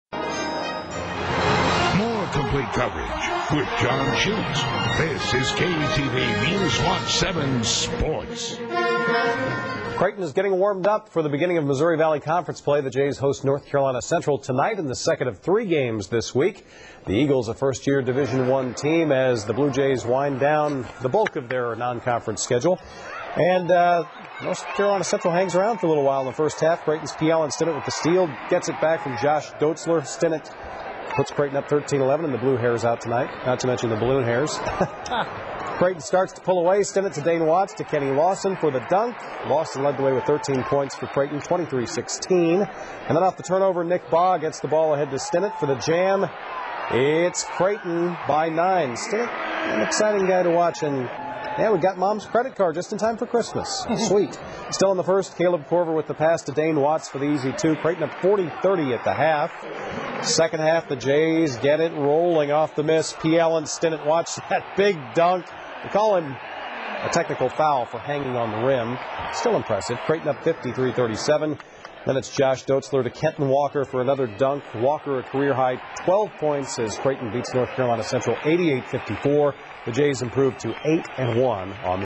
Creighton Gets Easy Win
The Bluejays are in action at Qwest.